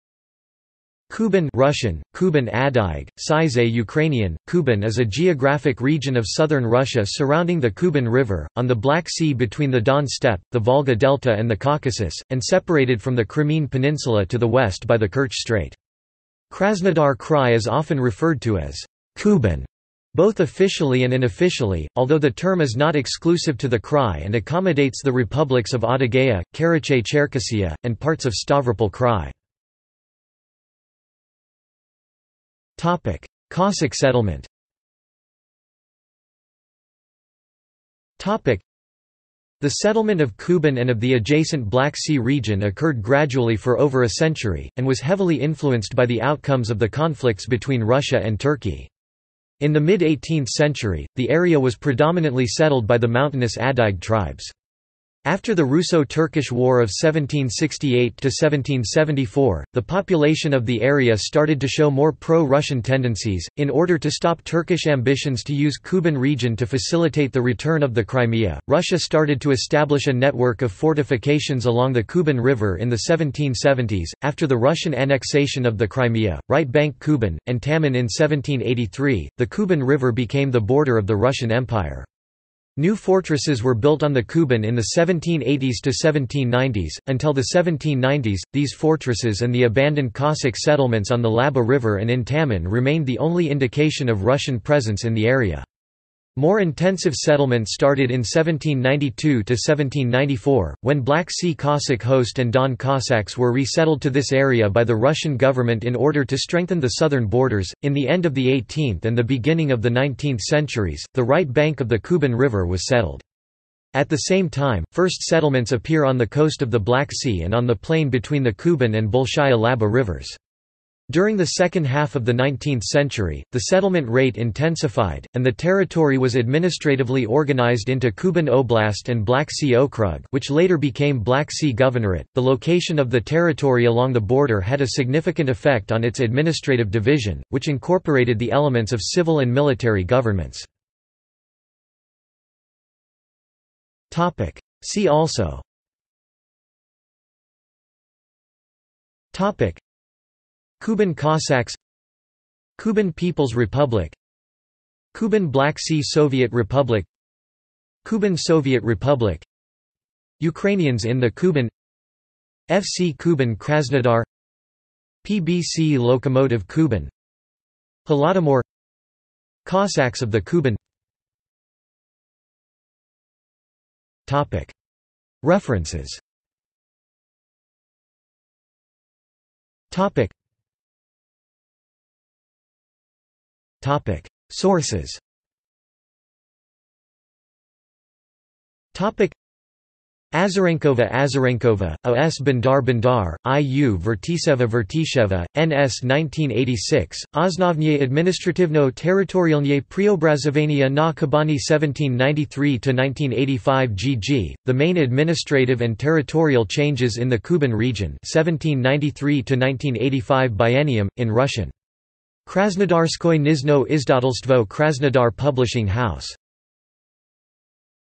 Kuban | Wikipedia audio article
This is an audio version of the Wikipedia Article:
Kuban

SUMMARY
Kuban (Russian: Кубань; Adyghe: Пшызэ; Ukrainian: Кубань) is a geographic region of Southern Russia surrounding the Kuban River, on the Black Sea between the Don Steppe, the Volga Delta and the Caucasus, and separated from the Crimean Peninsula to the west by the Kerch Strait. Krasnodar Krai is often referred to as "Kuban", both officially and unofficially, although the term is not exclusive to the krai and accommodates the republics of Adygea, Karachay-Cherkessia, and parts of Stavropol Krai.